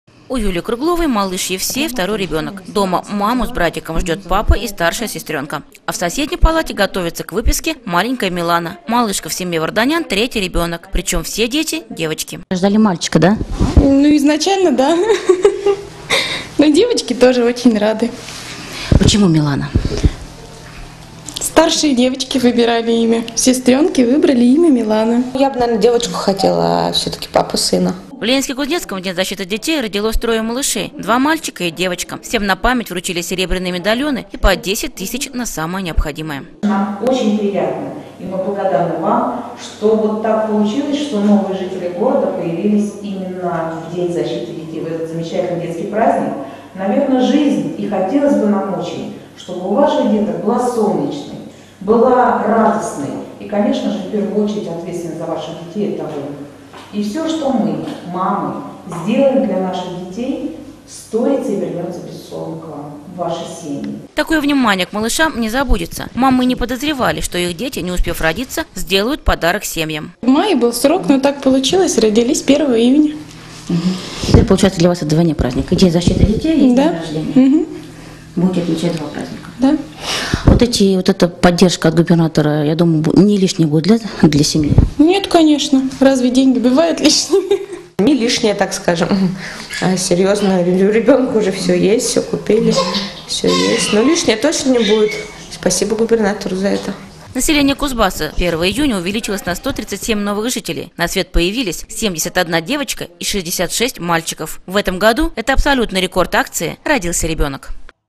Рожденные в День защиты детей
По  кузбасской традиции,  мамам  малышей,  рождённых  в  международный  день  защиты  детей,  вручили  по  10  тысяч  рублей  и  серебряные  медальоны  «Рождённому  на  земле  Кузнецкой».  Напомним,  что  по  решению Амана Тулеева,  в  регионе  с  2000-го  года  действует  акция  «Родился  ребёнок»,  и  пять  раз  в  год:  в  День  Матери,  на  Новый  год  и  Рождество,  8  Марта  и  в  День  защиты  детей,  новорождённые  получают  подарки.